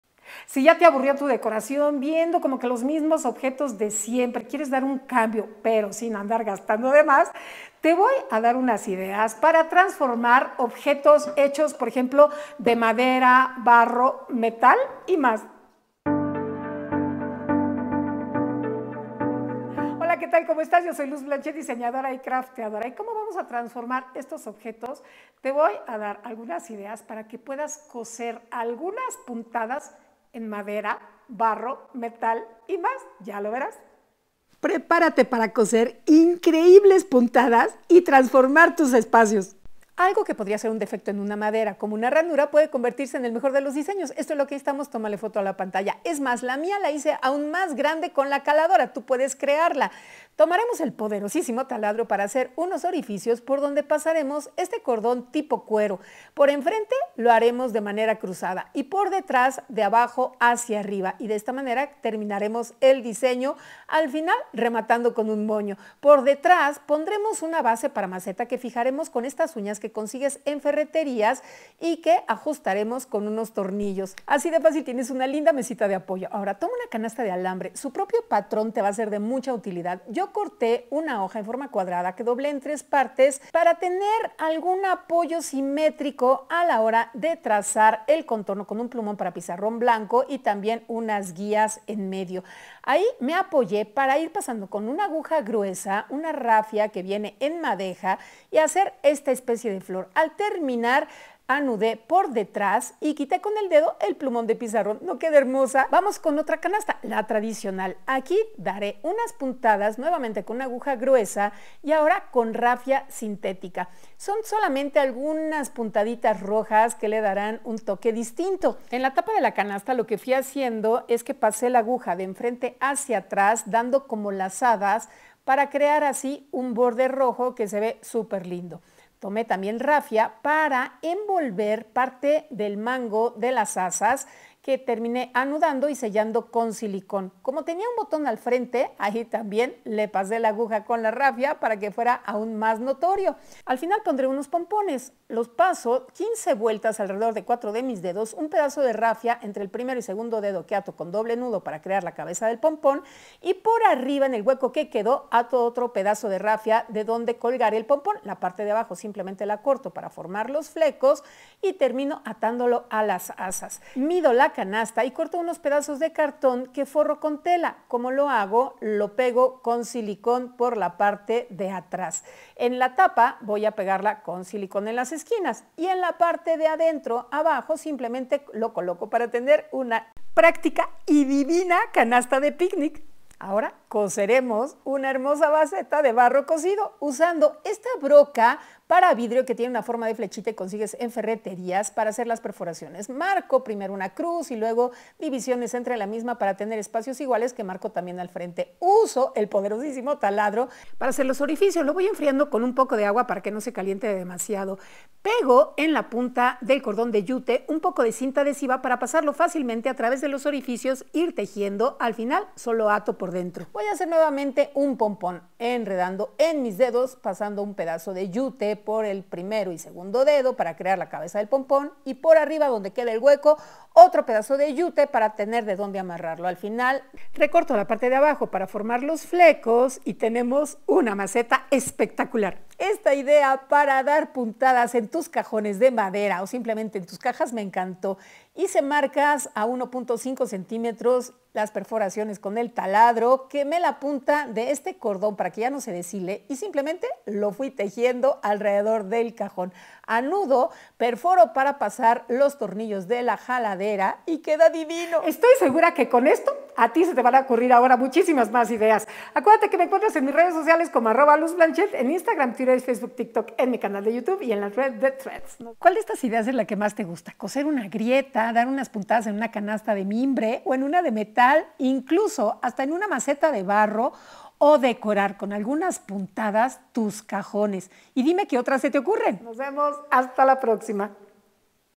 SI! Puedes COSER Puntadas FÁCIL en MADERA, BARRO, METAL y transformar tu DECORACIÓN / Luz Blanchet
Lo creías imposible? Es posible coser en madera, barro, metal y más unas sencillas puntadas y cambiar por completo tus objetos para transformar tu decoración!!!